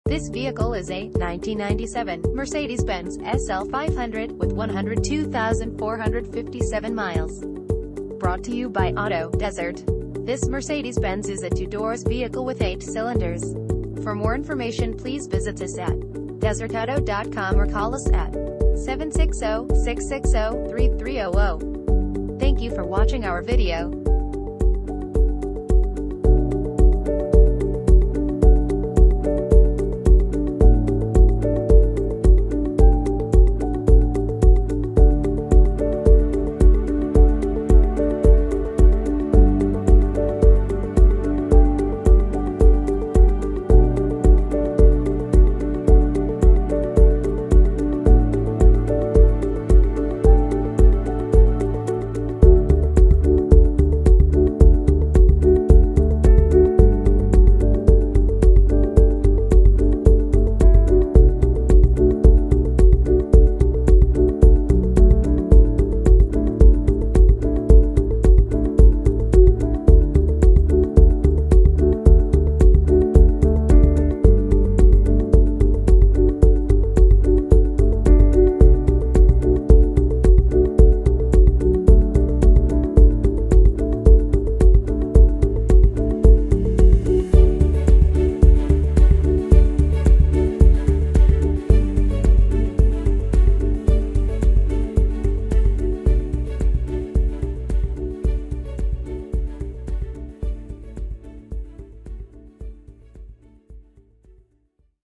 1997 Mercedes-Benz SL500 - Auto Desert
Auto Desert
74990 Joni Dr Unit A1

1997 Mercedes-Benz  SL500

2 owners, Clean title, Extended Warranty Available, 102,457 miles, Automatic, Teal exterior, Convertible, RWD: Rear Wheel Drive, 2 doors, 8 cylinders, Gasoline, MPG City 15 and 23 in Hwy.